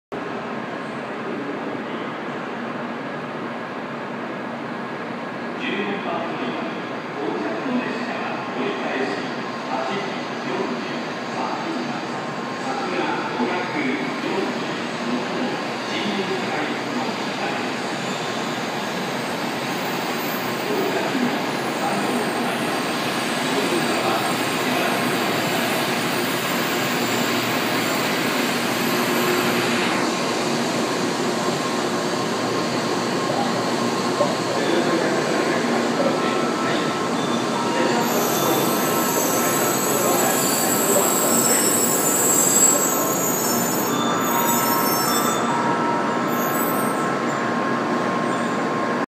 九州新幹線 さくら 546号 鹿児島中央駅に入線 N700系
九州新幹線の車窓動画は、以下でまとめております。